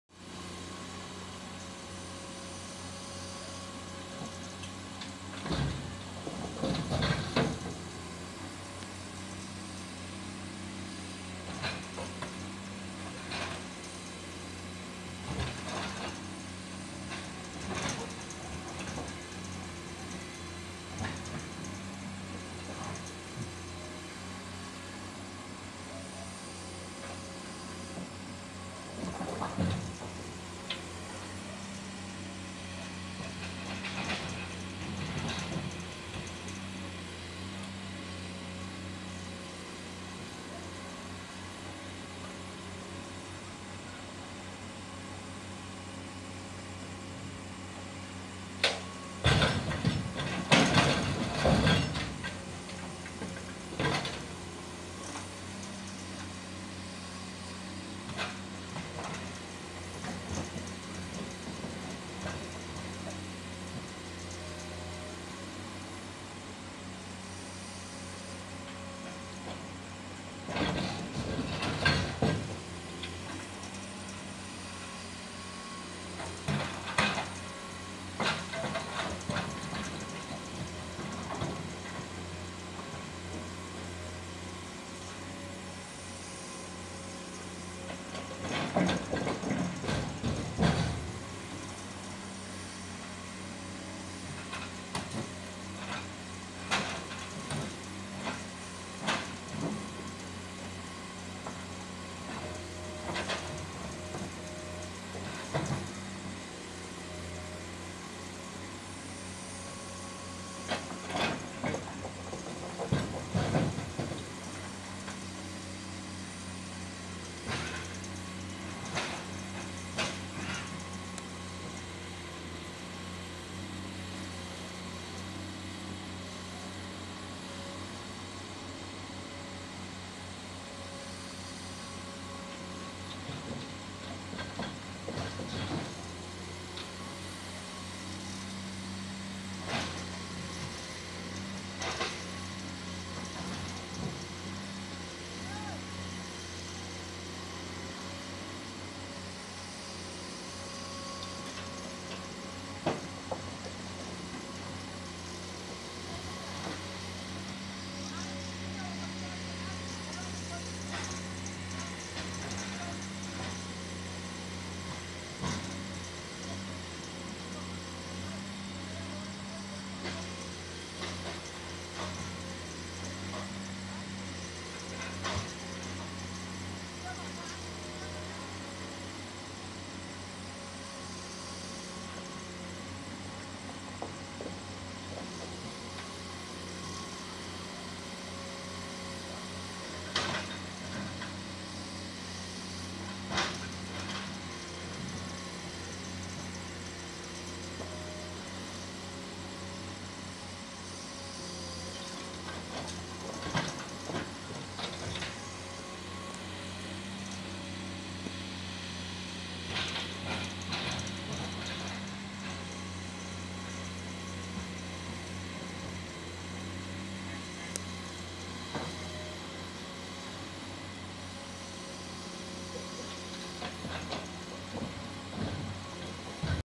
hydraulic excavator over time loading
Overtime sa pag loading ng mga Rock armour